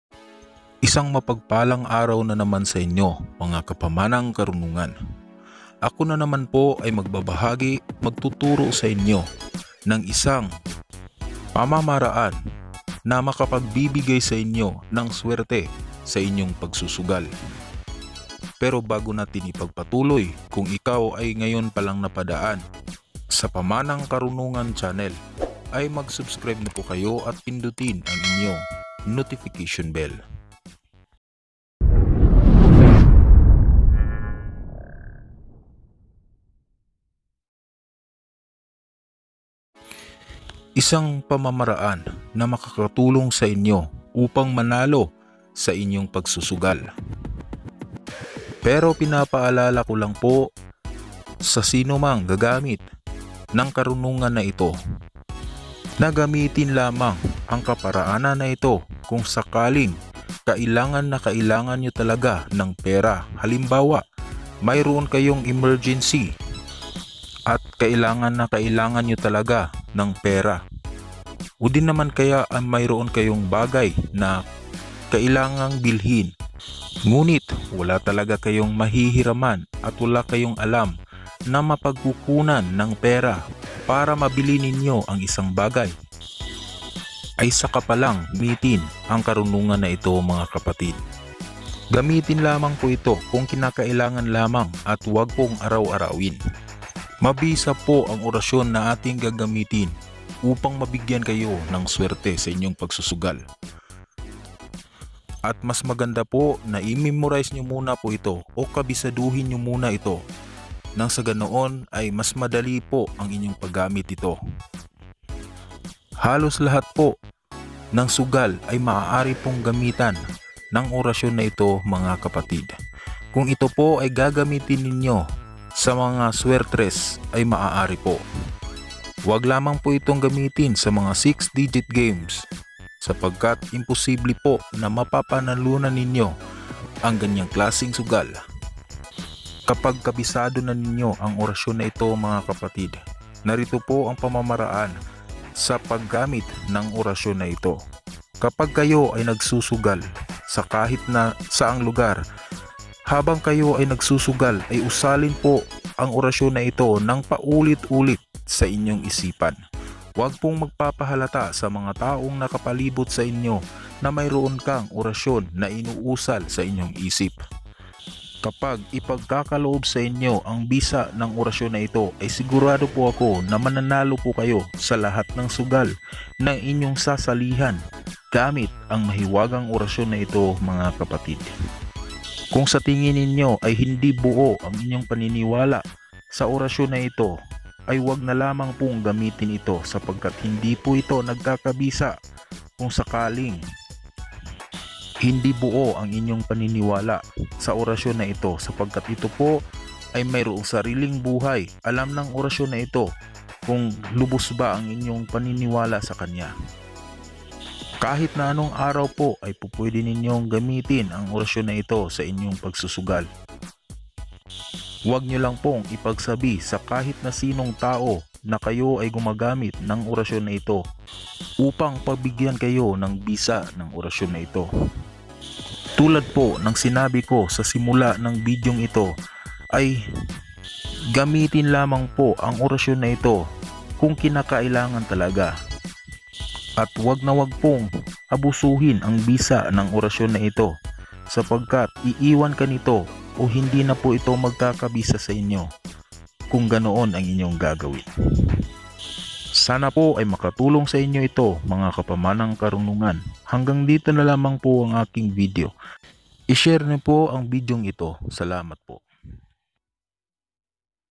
Orasyon para manalo sa sugal
Ito ay isang mabisang orasyon upang manalo ka sa sugal..